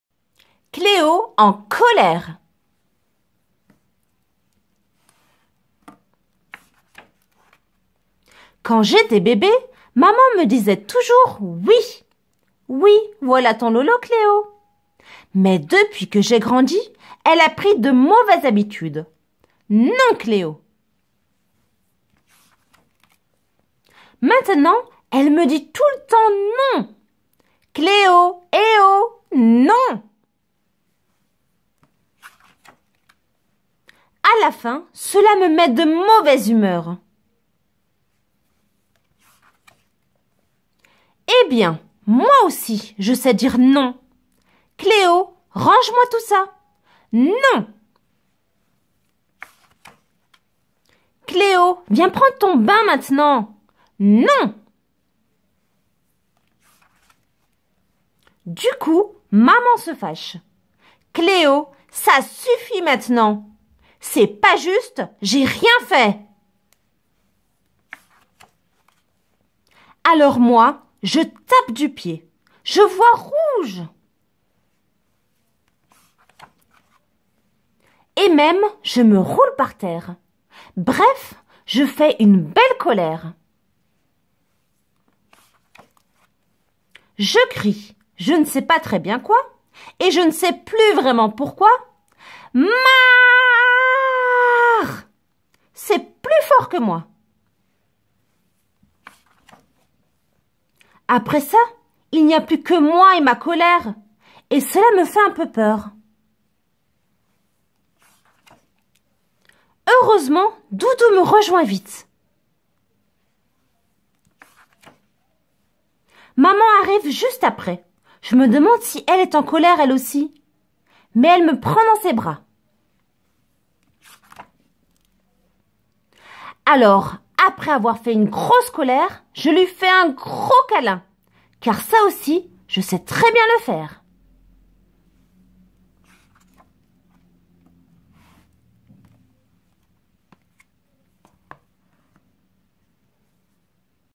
Cléo en colère- Lecture à haute voix 001-Fondation Toya
La LECTURE A HAUTE VOIX est une pratique essentielle pour le renforcement des capacités des filles des LIT CLUBS de la Fondation Toya.

Pour notre première séance de lecture à haute voix sur notre chaine YouTube, nous vous invitons à apprécier cette courte histoire: Cléo en colère

Les enfants comme les plus agés s'amuseront.

Pour être mieux imbus de nos activités, restez comnectés a nos différents réseaux:

N.B: Cette vidéo est une rédiffusion sur la page de la Fondation Toya